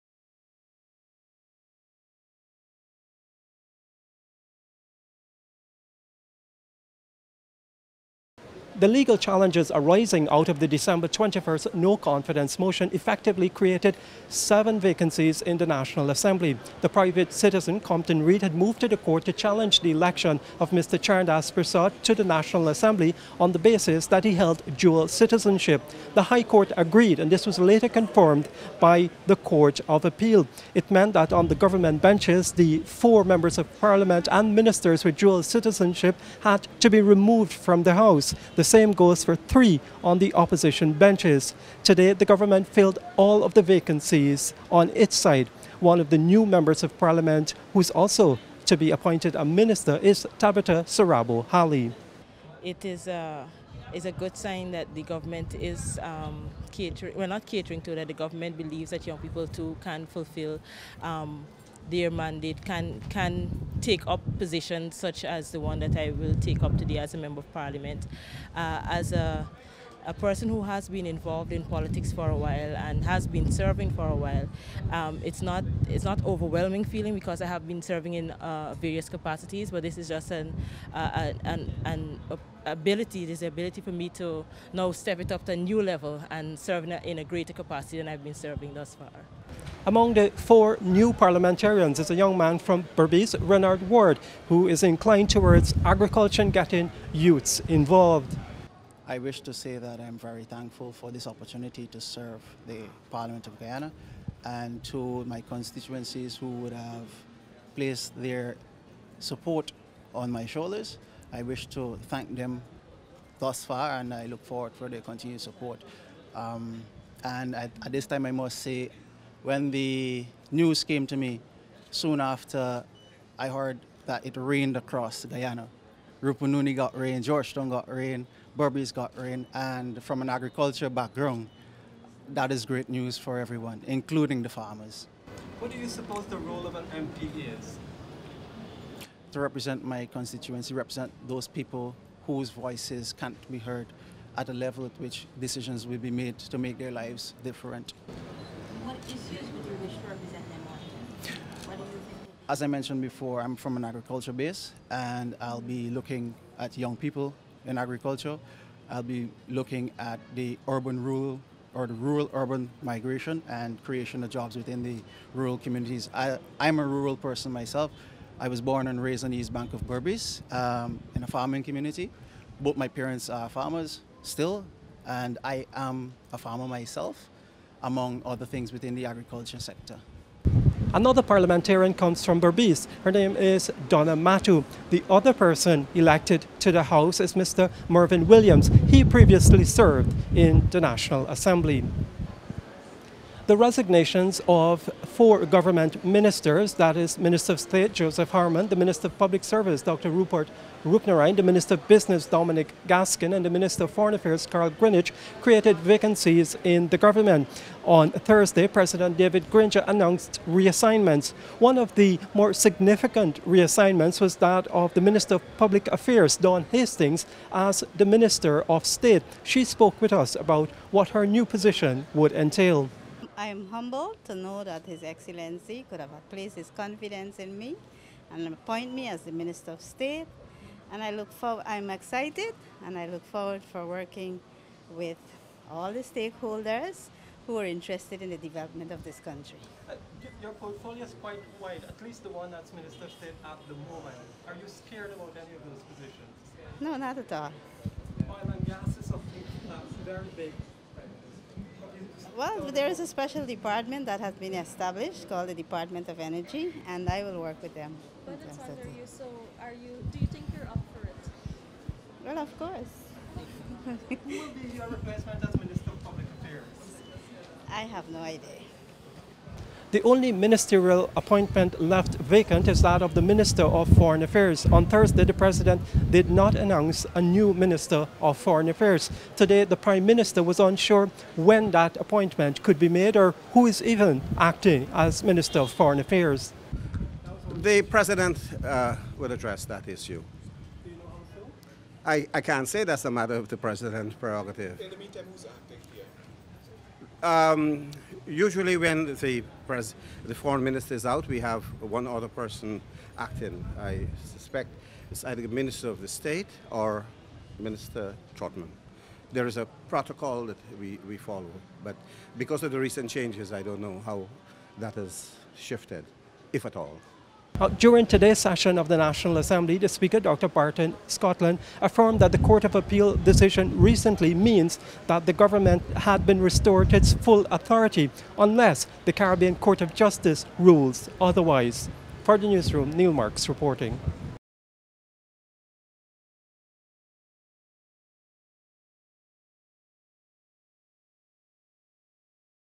NEW PARLIAMENTARIANS SWORN IN
The National Assembly on Friday held its first sitting after rulings by the High Court and the Court of Appeal on the December 21 No Confidence motion. The News Room’s Neil Marks was at the National Assembly, where new Parliaments and those Ministers reassigned responsibilities spoke about their new appointments for the first time. Neil Marks joins us…